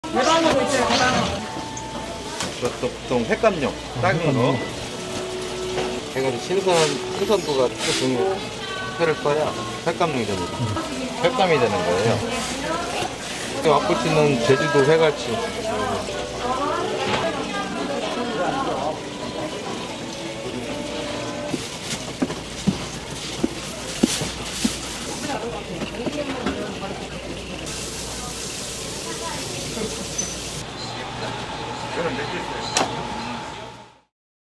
갈치를 회로 먹는다? 일명 풀치! 싱싱한 갈치회 썰기 달인! - Korean cutlassfish cutting skill master - street food
🎥 갈치를 회로 먹는다? 일명 풀치! 싱싱한 갈치회 썰기 달인! - Korean cutlassfish cutting skill master - Korean street food

🧡 Information 🧡

🟠[위치]
- 동문수산시장
[Google map]

제주특별자치도 제주시 이도1동 관덕로 64-1